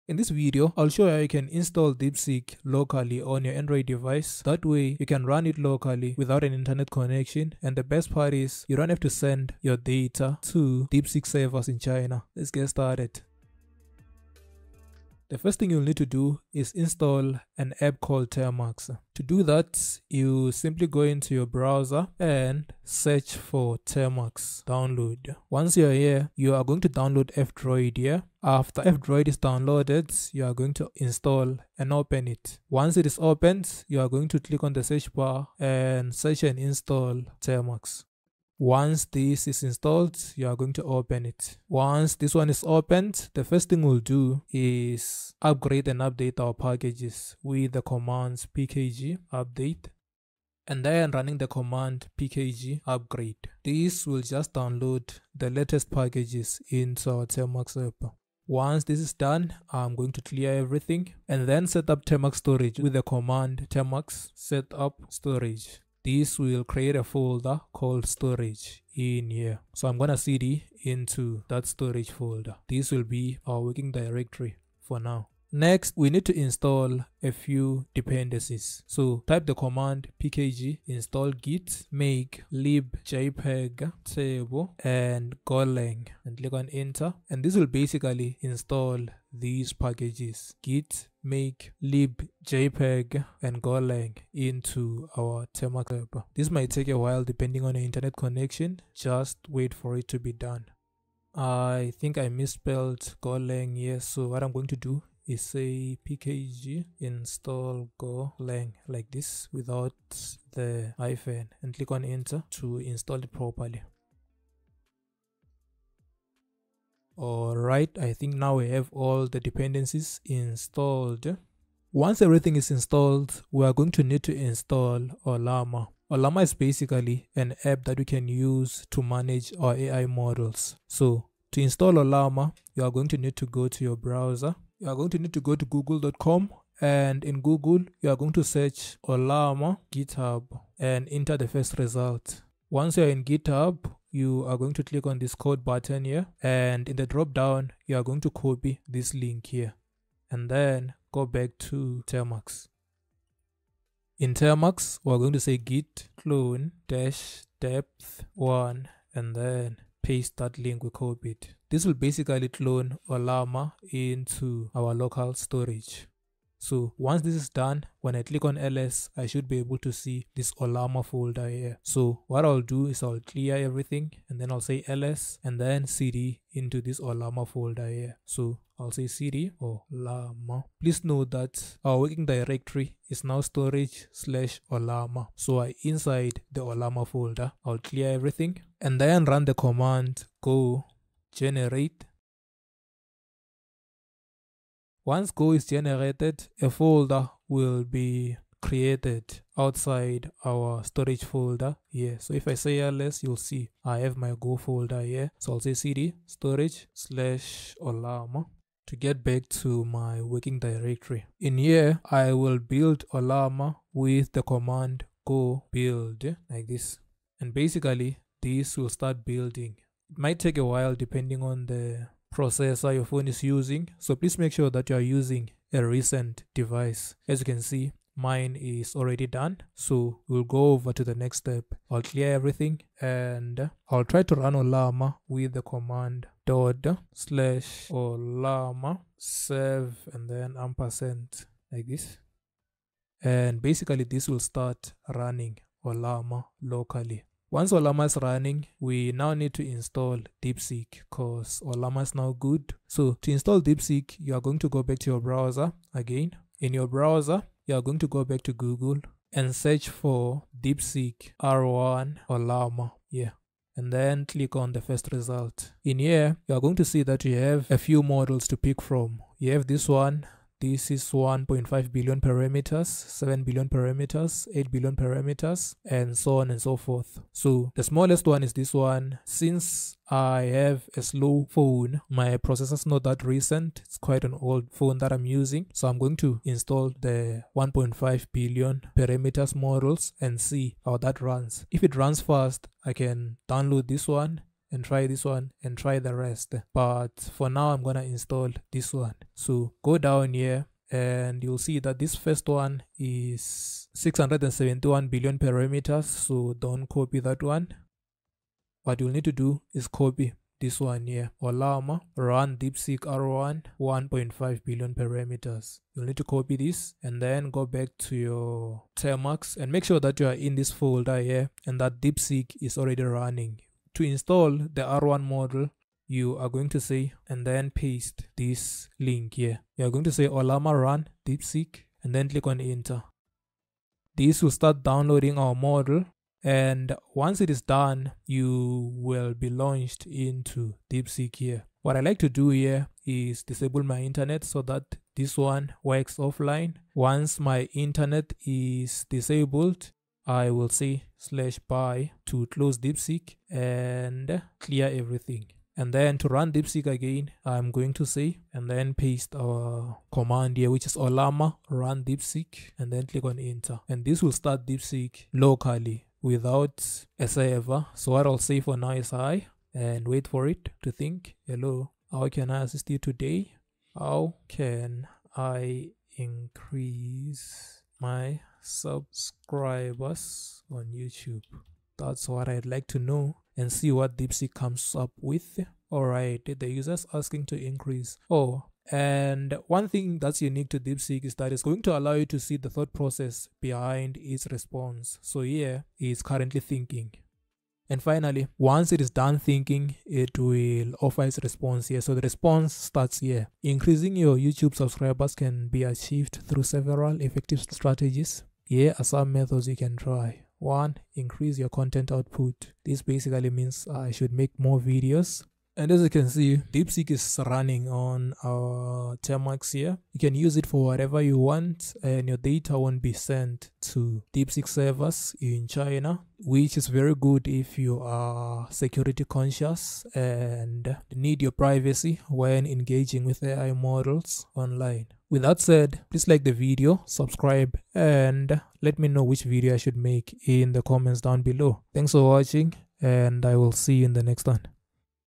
How to Run Deepseek Locally on Android 🔥
In this tutorial, I'll show you step-by-step how to install and run DeepSeek locally using Termux. You'll learn how to set up Termux, install dependencies, build Llama, and download DeepSeek models—all without an internet connection after setup. Keep your privacy intact while running AI models directly on your device!

🕒 Timestamps:
0:00 - Introduction & Why Run DeepSeek Locally
0:15 - Step One: Install Termux
1:15 - Installing Dependencies (Git, Make, Golang, etc.)
1:58 - Step Two: Install Ollama
4:05 - Step Three: Cloning & Building Llama in Termux

Happy coding! 🔧💻